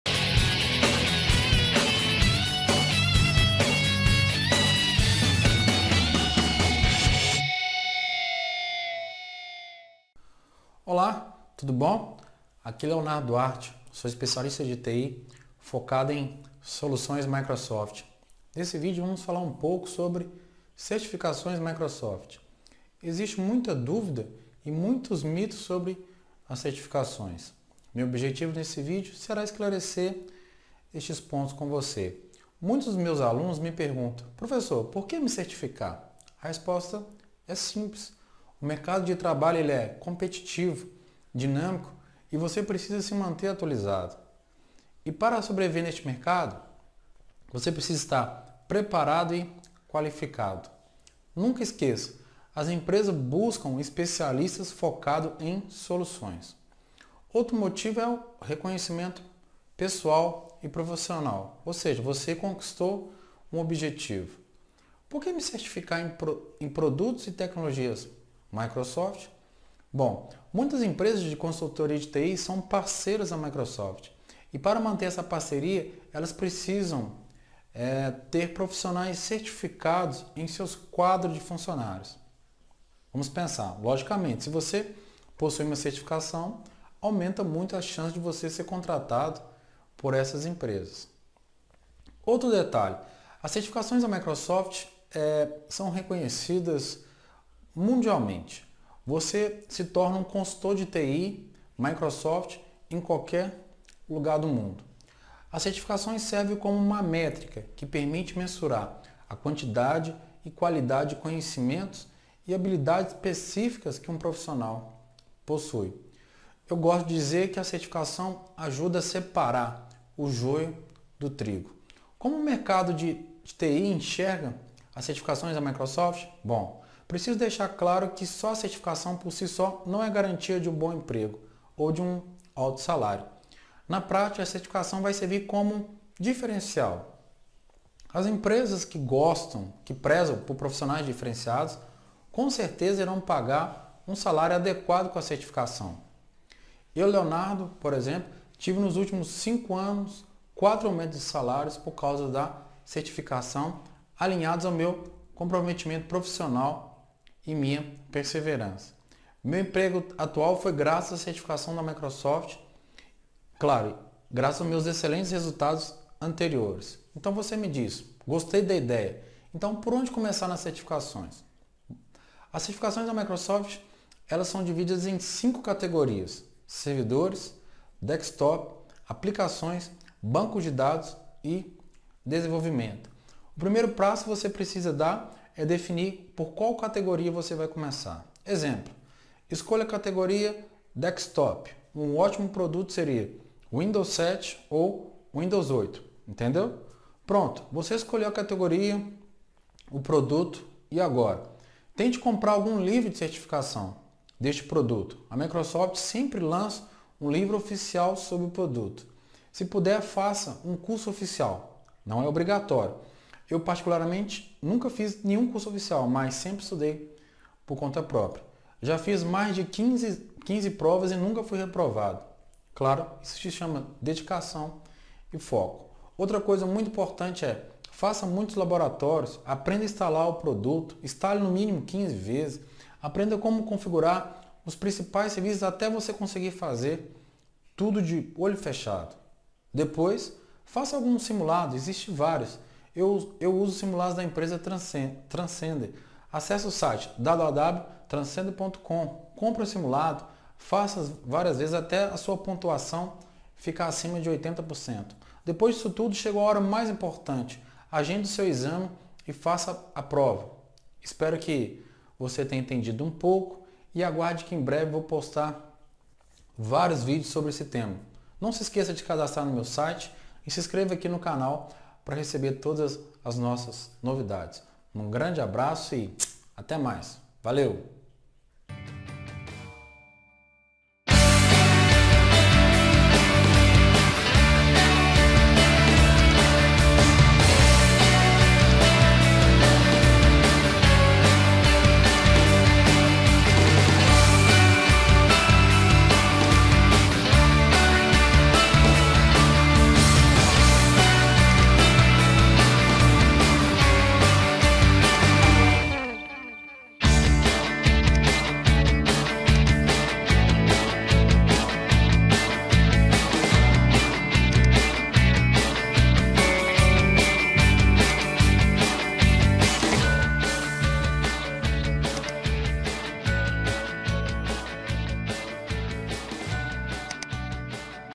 🔴 Porque Tirar Uma Certificação em Tecnologia
▬▬▬▬▬▬▬▬▬▬▬ஜ۩۞۩ஜ▬▬▬▬▬▬▬▬▬▬▬▬▬
▓▓▓▒▒▒░░░ LEIA A DESCRIÇÃO DO VÍDEO ░░░▒▒▒▓▓▓
▬▬▬▬▬▬▬▬▬▬▬ஜ۩۞۩ஜ▬▬▬▬▬▬▬▬▬▬▬▬▬

Neste video falo da importância de ser um profissional certificado e abordo sobre as Certificações Microsoft.